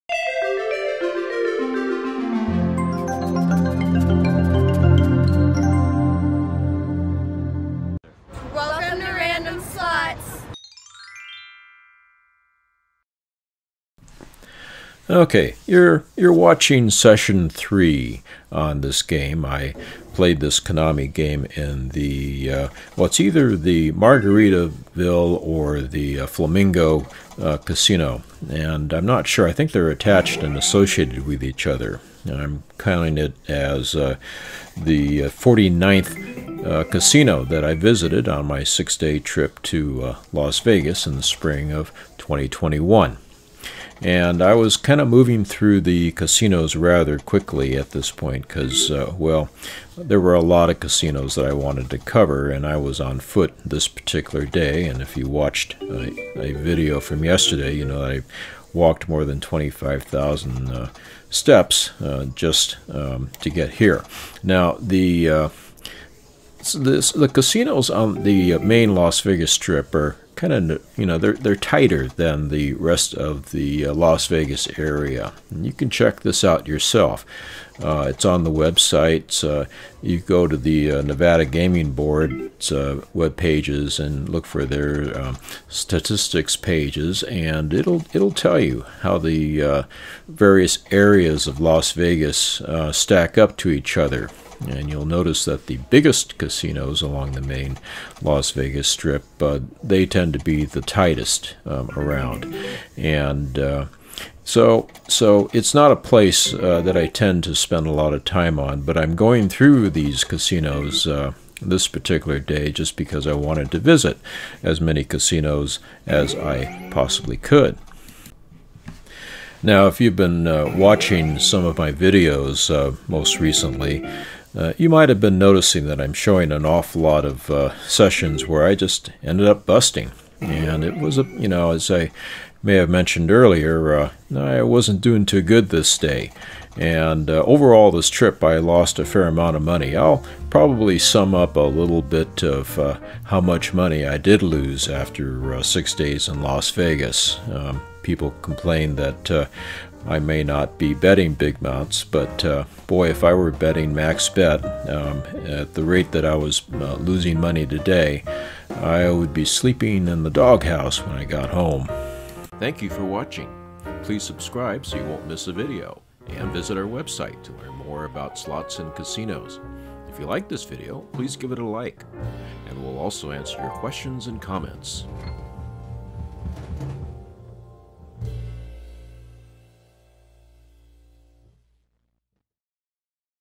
📈 Exotic Princess Slot Machine & Strip Casino Statistics
Played at / Slot Brand: Flamingo Vegas (Margarita?) / Konami
When: March 2021
How Many Casinos Did I Play Slots in During this 6 Day Trip to Vegas?
The number of casinos so far is indicated in the "Las Vegas" logo in the lower right of the thumbnail